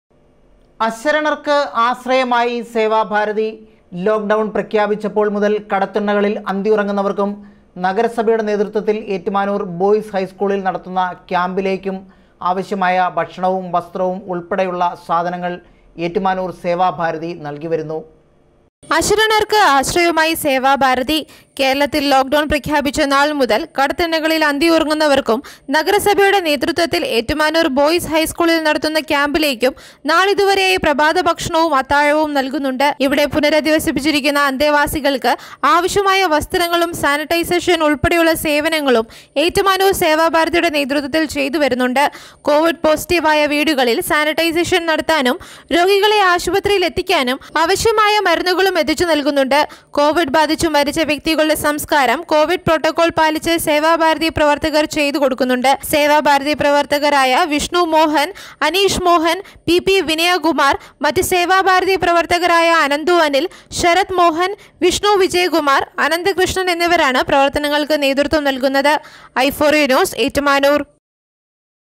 അശരണര്‍ക്ക് ആശ്രയമായി സേവാഭാരതി
അശരണര്‍ക്ക് ആശ്രയമായി സേവാഭാരതി. ലോക് ഡോണ്‍ പ്രഖ്യാപിച്ച നാള്‍മുതല്‍ കടത്തിണ്ണകളില്‍ അന്തിയുറങ്ങുന്നവര്‍ക്കും, നഗരസഭയുടെ നേതൃത്വത്തില്‍ ഏറ്റുമാനൂര്‍ ബോയ്‌സ് ഹൈസ്‌കൂളില്‍ നടത്തുന്ന ക്യാമ്പിലേക്ക് ആവശ്യമായ ഭക്ഷണവും, വസ്ത്രവും  ഉള്‍പ്പെടെയുള്ള സാധനങ്ങള്‍ ഏറ്റുമാനൂര്‍ സേവാഭാരതി നല്‍കിവരുന്നു.